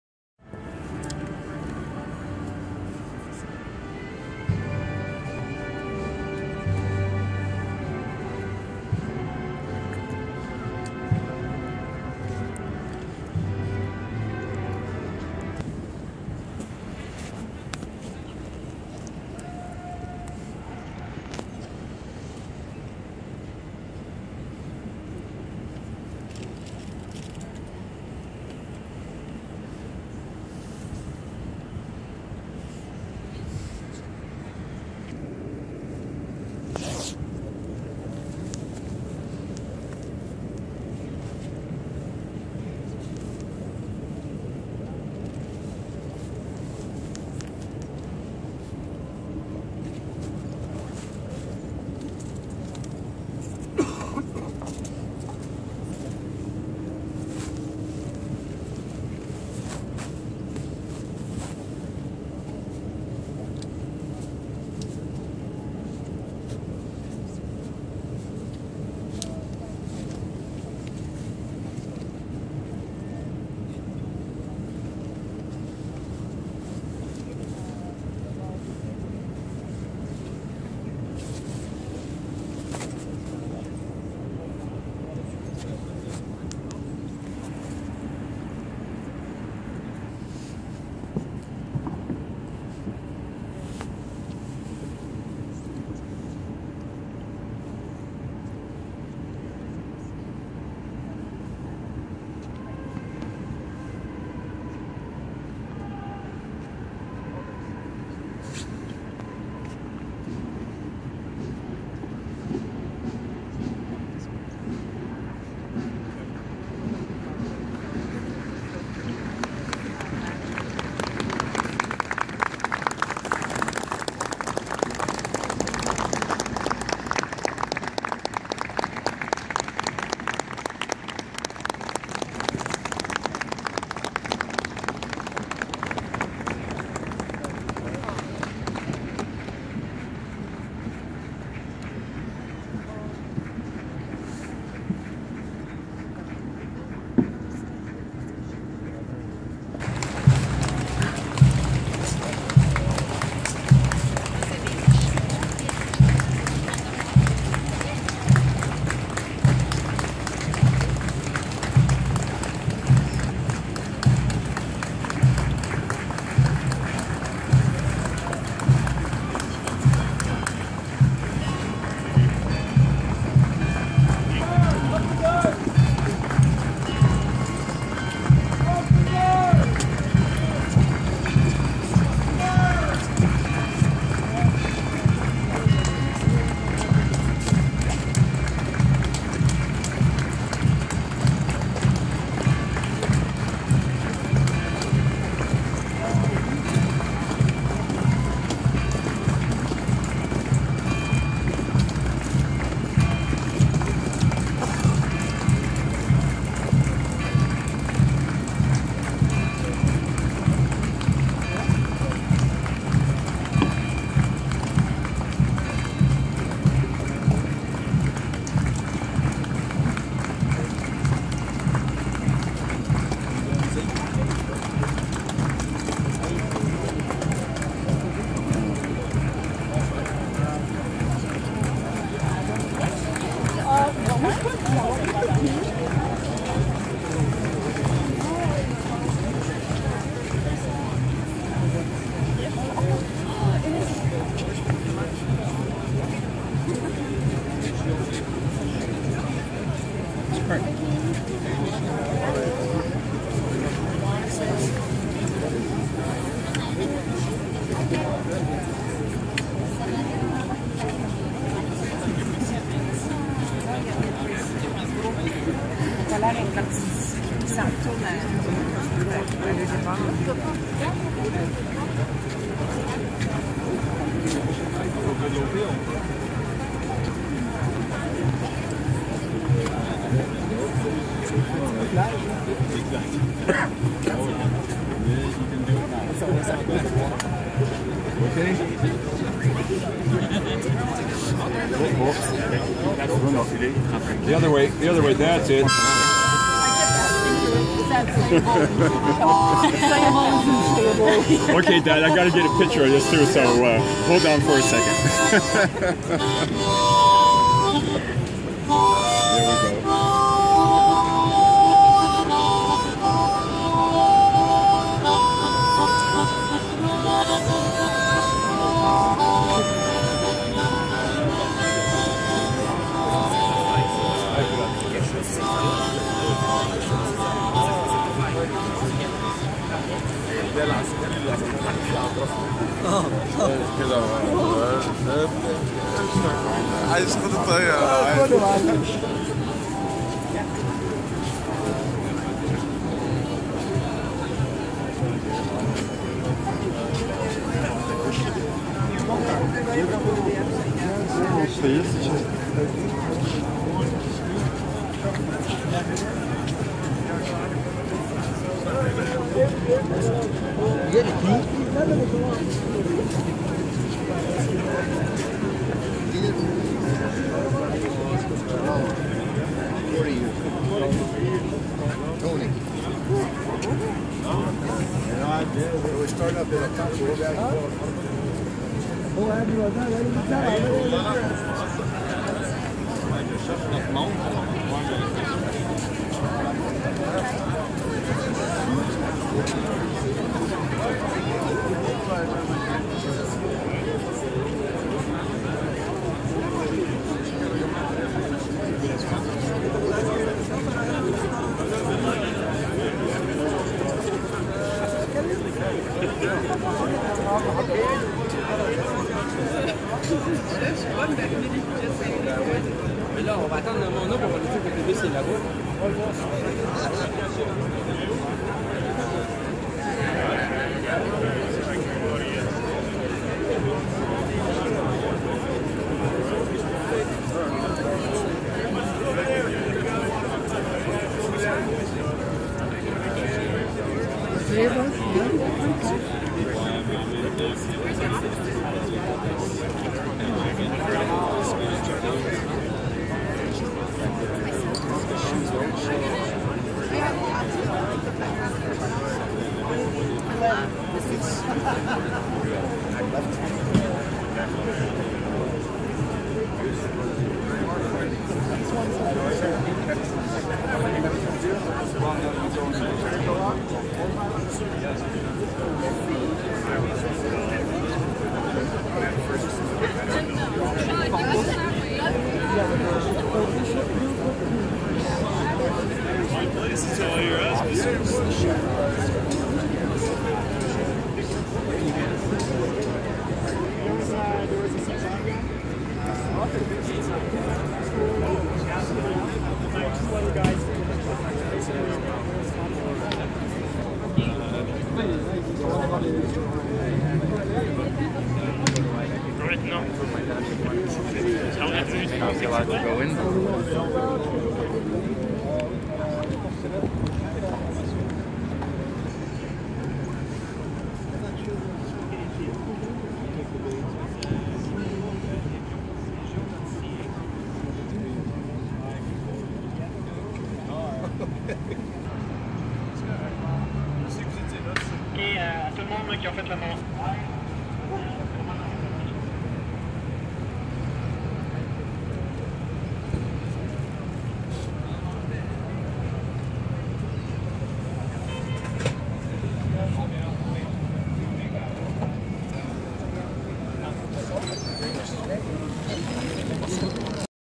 Remembrance Day 2014 The End & Harmonica
Remembrance Day 2014
Montreal, McGill
Just random images of people leaving
Plus a very cute old gent playing the harmonica 4:56